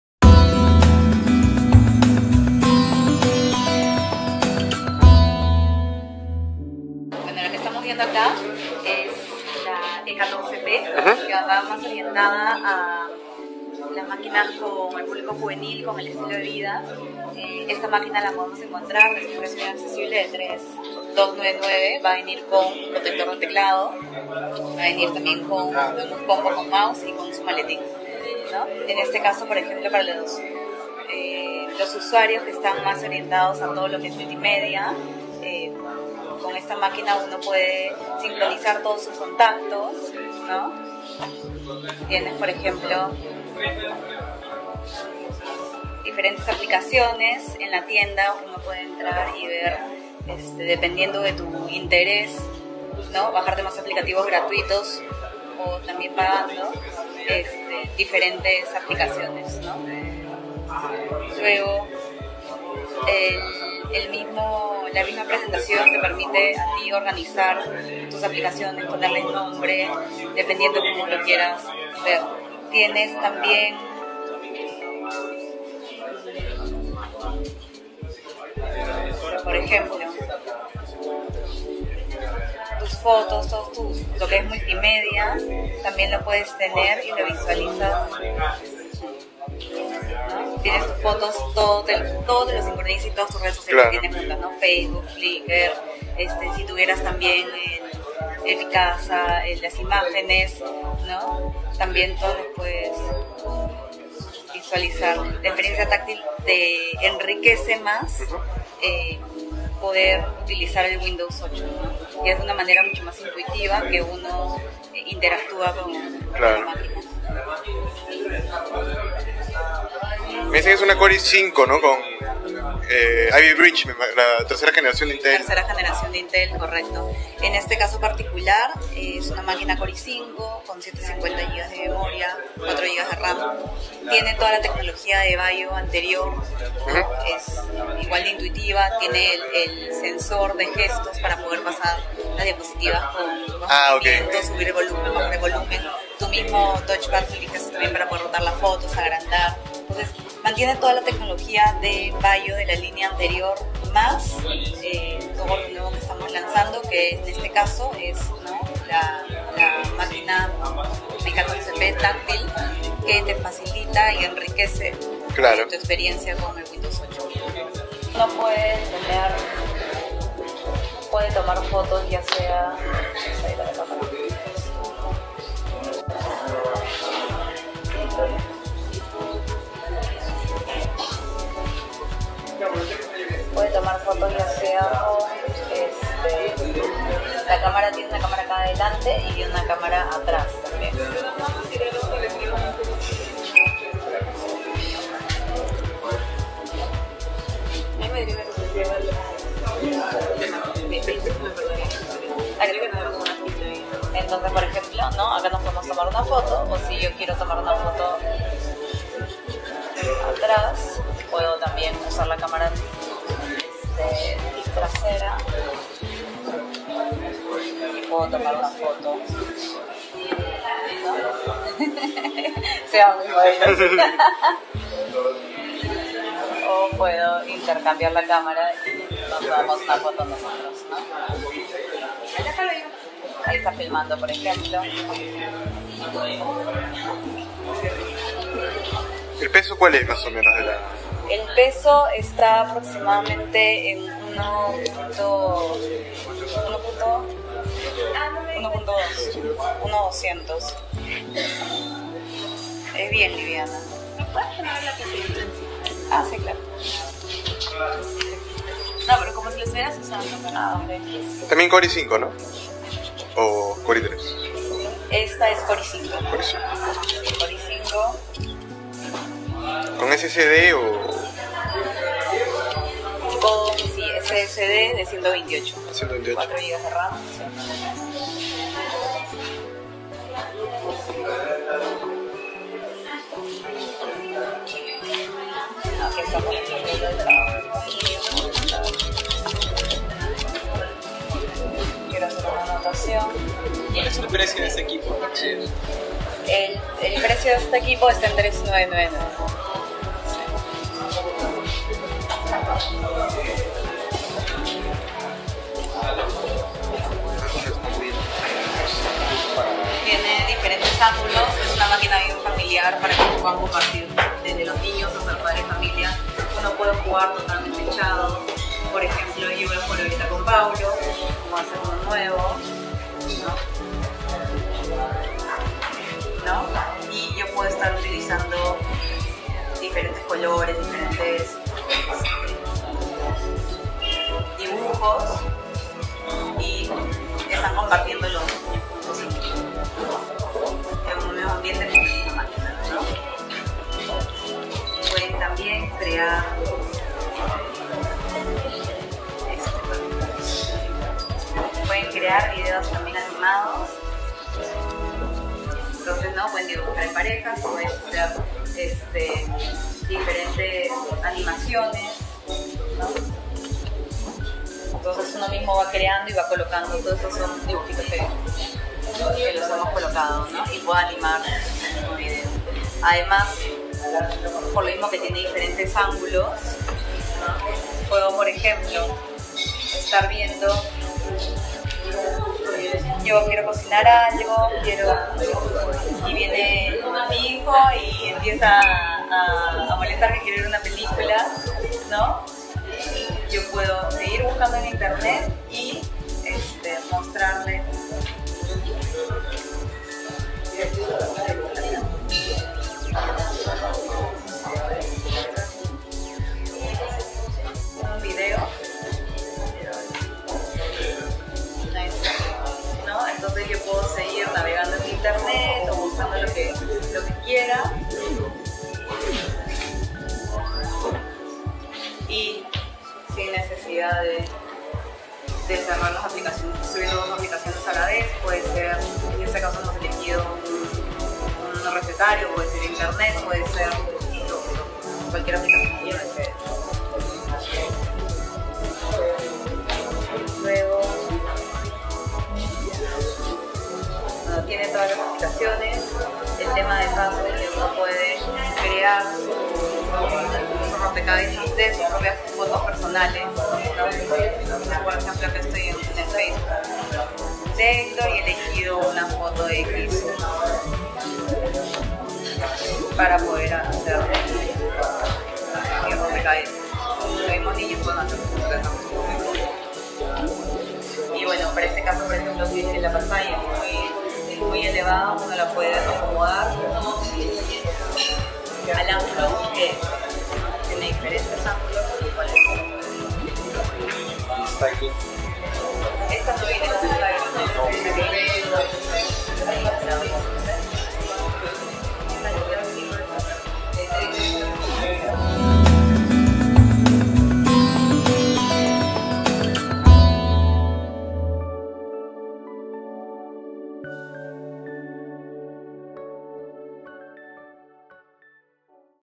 Sony Vaio Touch en Lima- Vaio Duo 11, Tap 20, E14P
Presentación oficial de la línea Vaio Touch con Windows 8 de Sony. Mas info en arturogoga.com